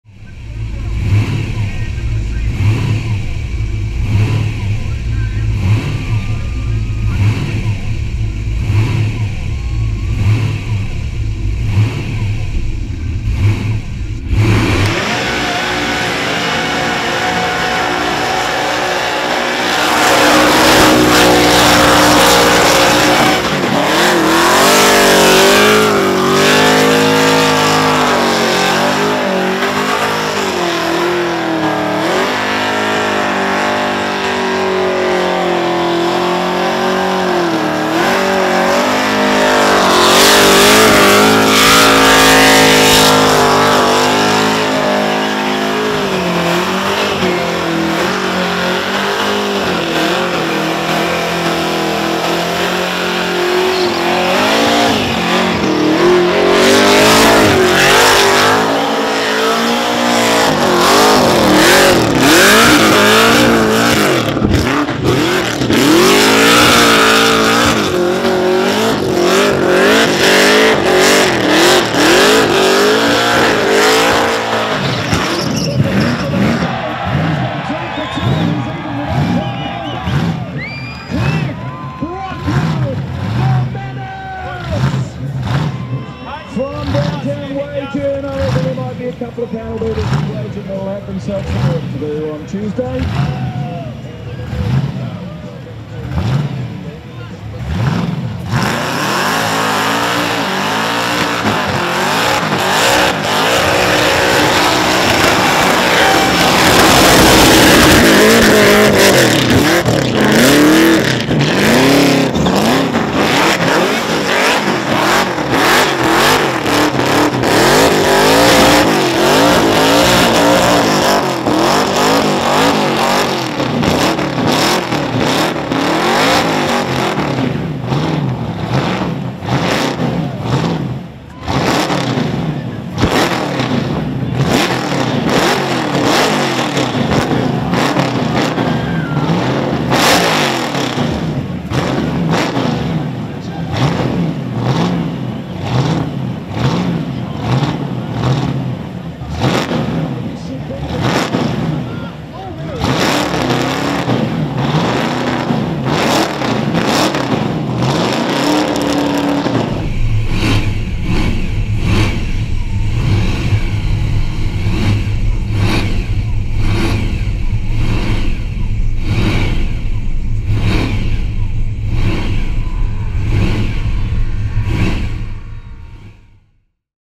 SUPERCHARGED MERCEDES "MENACE" TAGS THE WALL TWICE AT BURNOUT KING
Cliff Brockwell gets lost in the smoke at Burnout King and Tags the Wall Twice in his Supercharged Big Block Ford powered Mercedes.